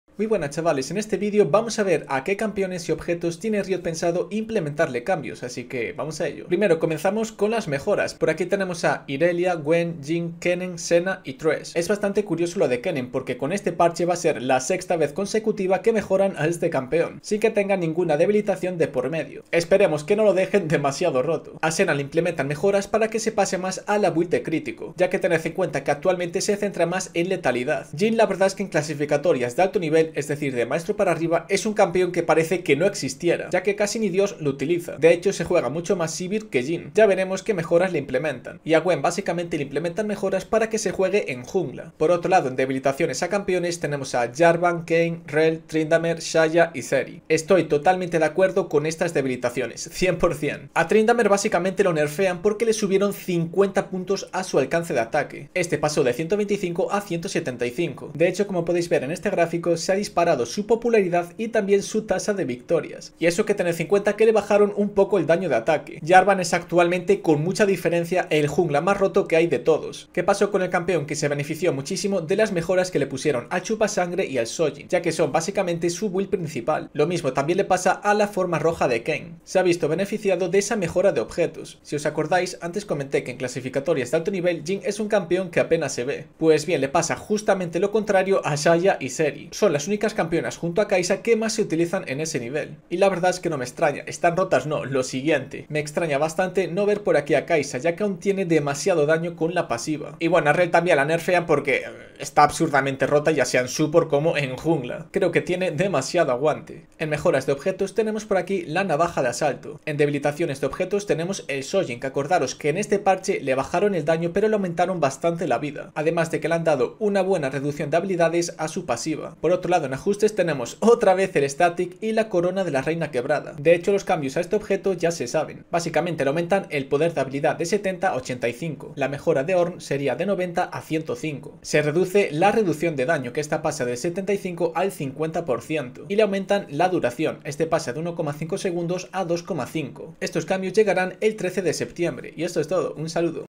FINALMENTE! Riot NERFEA a los 7 campeones más DESBALANCEADOS!
Vamos a ver a que campeones tiene Riot pensado implementar cambios con el parche 13.18 de League of Legends

0:00 Intro
0:06 Buff champs
0:41 Nerf champs
1:47 Buff items
1:50 Nerf items
1:59 Ajustes de items